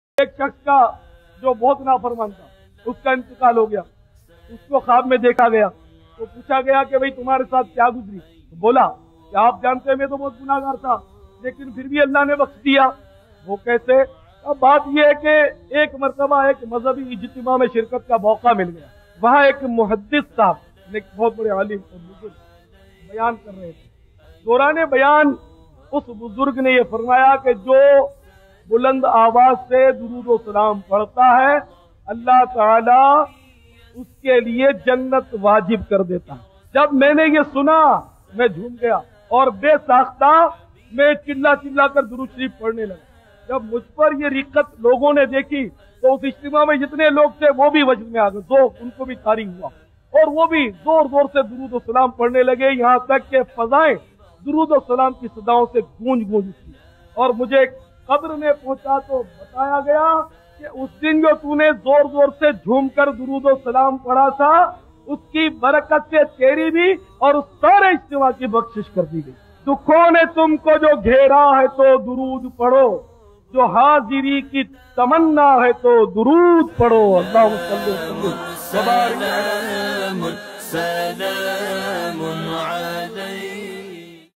Dawateislami | Aik Gunehgaar Shakhs Ki Maghfirat Kaise Hui ??? | Maulana Ilyas Qadri
ایک گنہگار شخص کی مغفرت کیسے ہوئی؟
جان کر آپ بھی خوشی سے جھوم جائیں گے...

¶ Dawateislami is one of the leading sunnah inspired spiritual channel of Pakistan